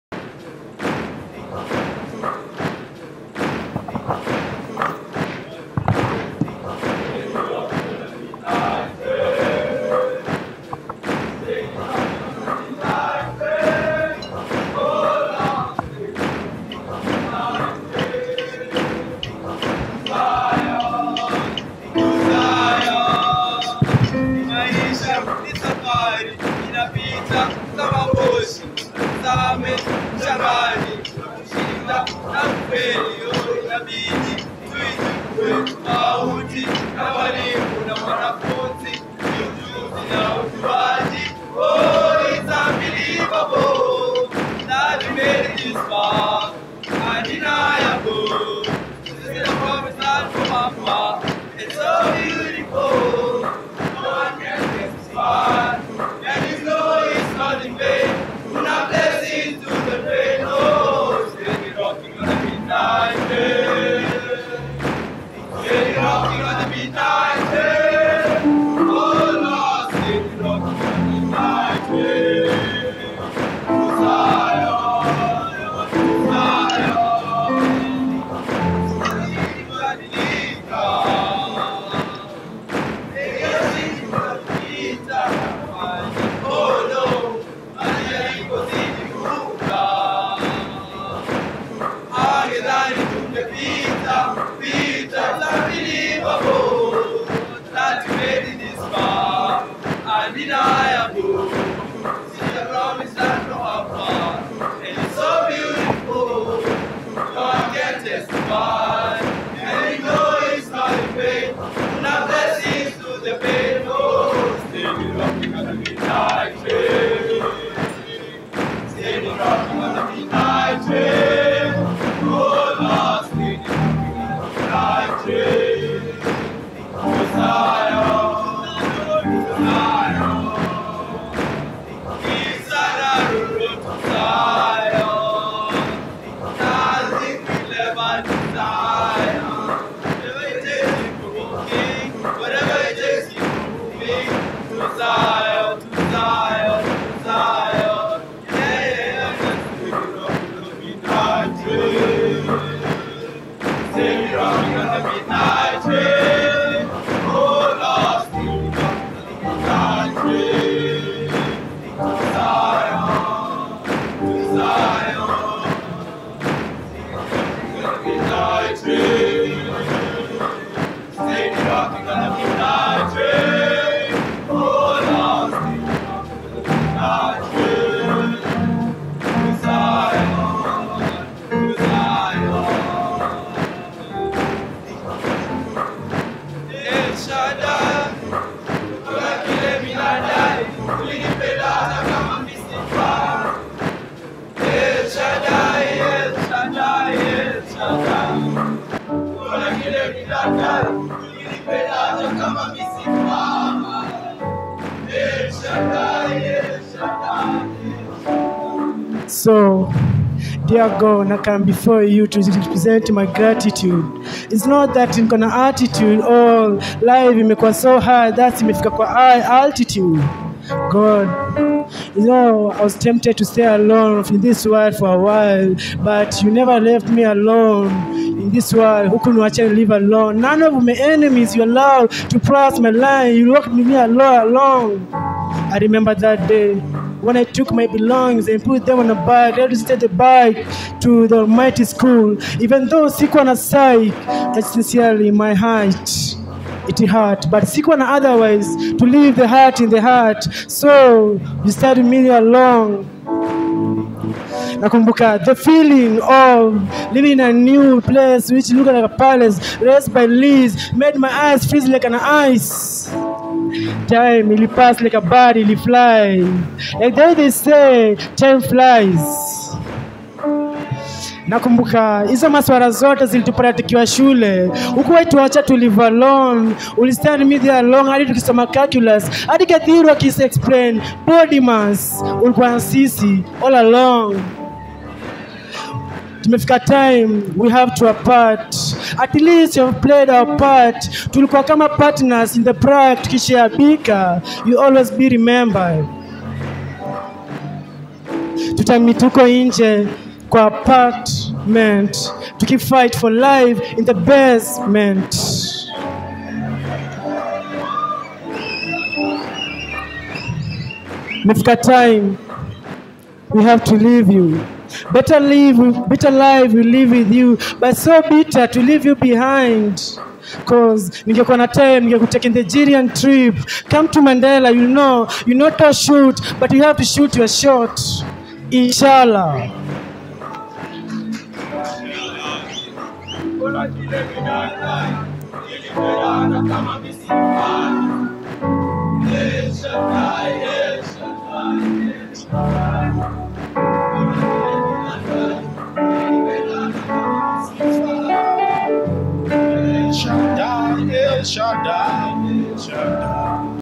MANDELA'S FINAL PERFORMANCE
A nostalgic moment for the Mandela student's as they summarise their journey in a song performance during the prayers day